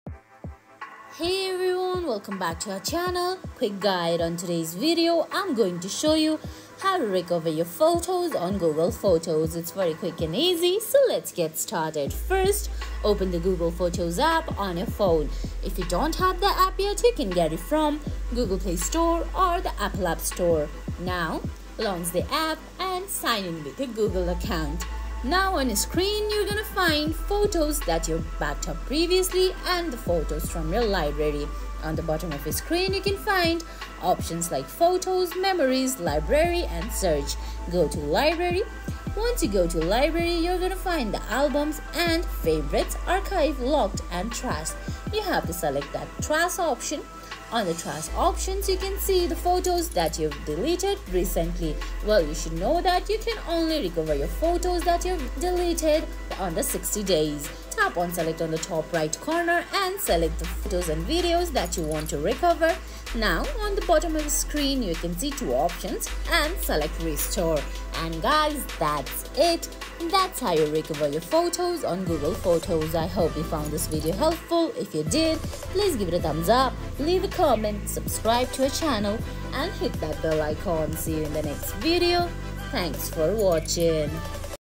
How to Recover Photos on Google Photos | Restore Deleted Images on Google Photos 2024
Accidentally deleted a photo on Google Photos and need to recover it? This guide will walk you through the process of restoring deleted photos from Google Photos. Learn how to navigate the Trash folder, recover lost images, and prevent future accidental deletions. Whether you’ve mistakenly removed important photos or need to retrieve images from a previous backup, we’ll help you restore your memories efficiently.